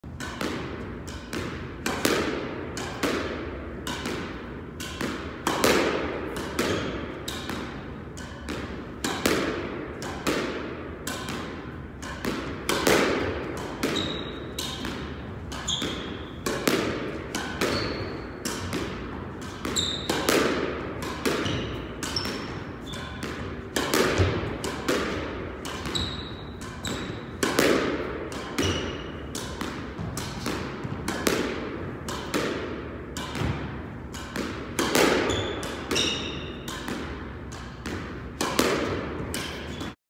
This Video Was Used To Create The FIRST Ever SQUASH NFT
Have you heard about the NFT (non-fungible token) hype going around the world? If not you should Google NBA Top Shots,  heck out GaryV or read the latest Serious Squash blog (SeriousSquash.com).

Yesterday I made the first ever squash NFT and I have listed it on Open Sea. It can only be a maximum of 30mb so it is just a fraction of this clip you are seeing here today.

So why wouldn’t anyone want to buy a copy of this video in a digital format on the blockchain? NFTs are basically the new version of collectables. Everything is going digital and so are collector items.

If you’re not into crypto you will probably have a hard time figuring out how to open a wallet and purchase any NFT. But if you are into crypto and you’re a squash fan this might be something of interest to you.

There are a maximum of 10 copies available for purchase at a price of .01 eth. I will be destroying the original version of this video so anyone who buys a copy of this first ever squash NFT will be 1 of only 10 to hold it. You can then resell or store it as a collectors item and something you can brag about to your other nerdy crypto squash buddies.

If you want to check it out you can find it listed